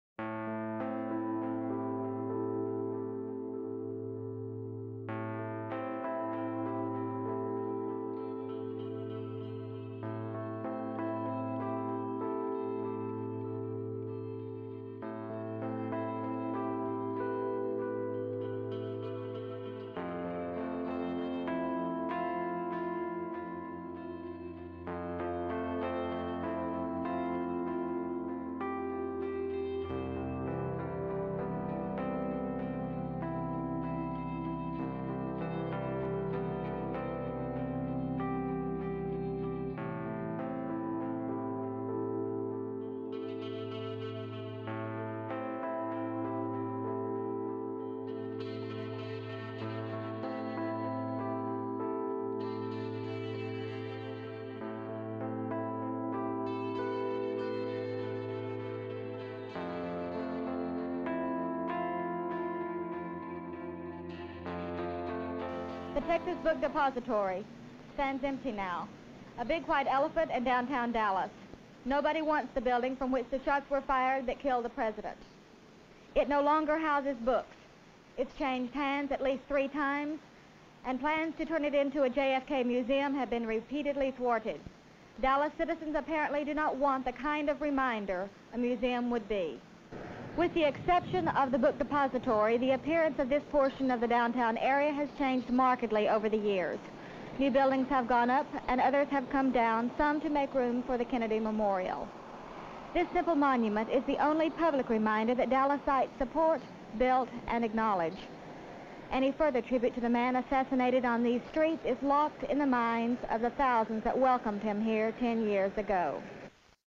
April 4, 1970 - Aubrey Mayhew purchases the infamous Texas School Book Depository at an auction
Aubrey Mayhew, a music producer from Nashville, was one of the nation's biggest collectors of 1960s JFK memorabilia, with more than 300,000 Kennedy related items in his possession. He was so obsessive about Kennedy, he purchased the infamous Texas School Book Depository at a 1970 auction in Dallas, Texas.
Also footage from 1973 - Tens years after the Assassination of President John F. Kennedy